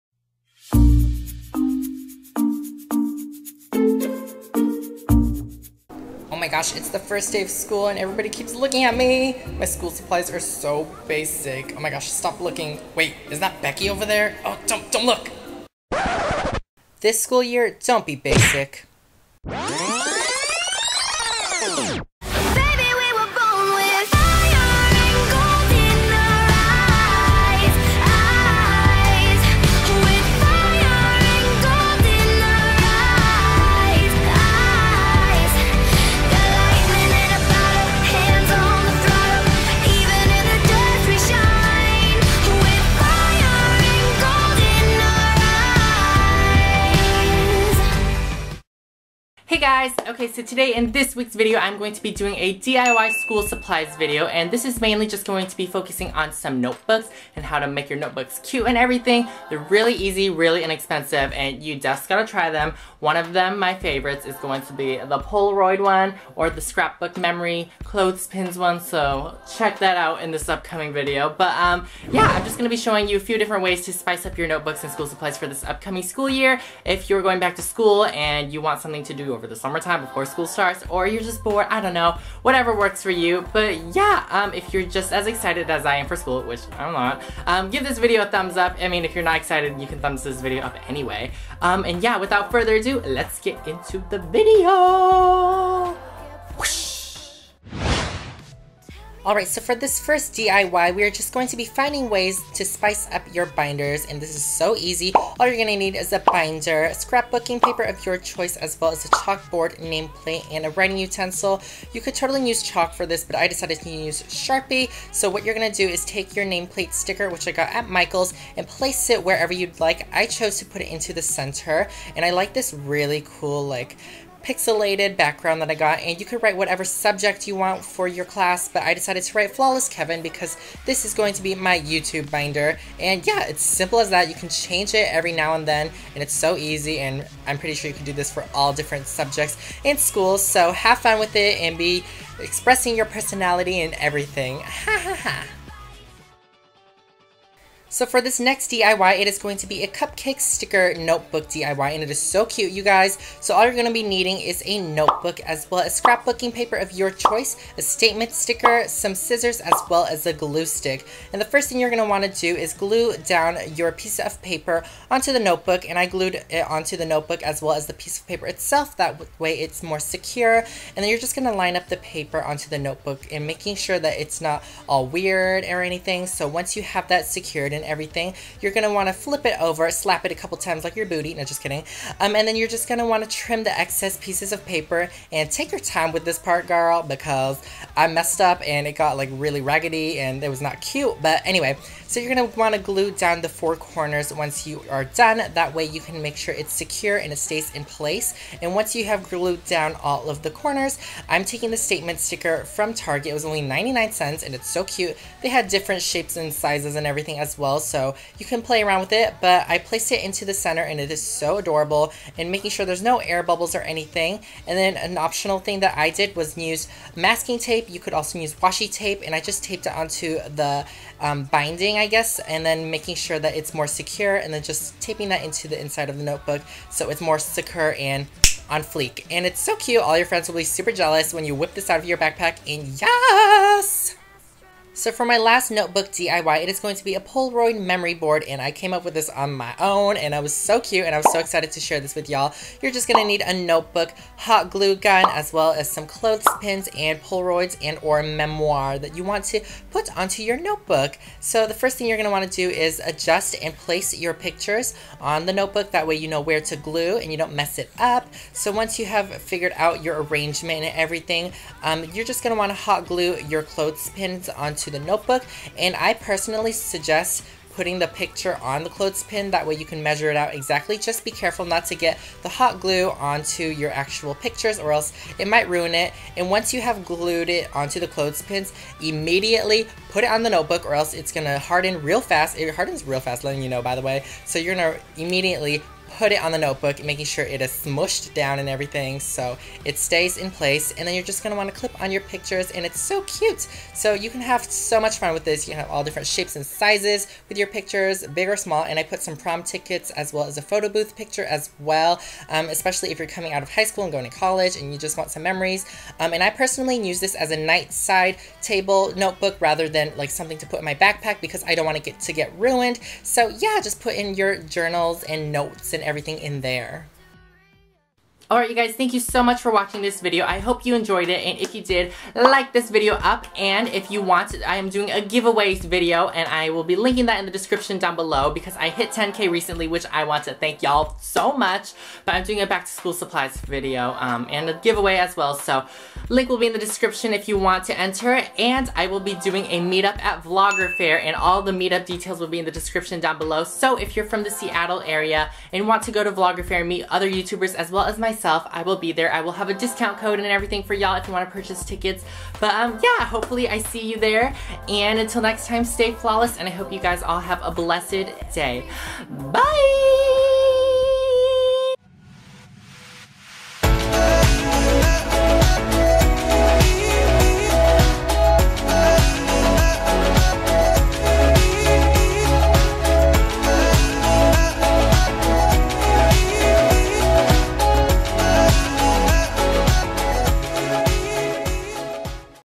DIY Tumblr Notebooks for Back to School!
Let's get this video up to 222 likes! This weeks video is another back to school video but this will be a DIY Back to School Supplies (DIY Notebooks)! Hopefully some of you start school later on but for those who already have, I hope y'all have a great school year! :)

MEETUP!
If you are around the Seattle area, you can purchase and find more info in the link above! See you at Vloggerfair! :)

Follow me @flawlesskevin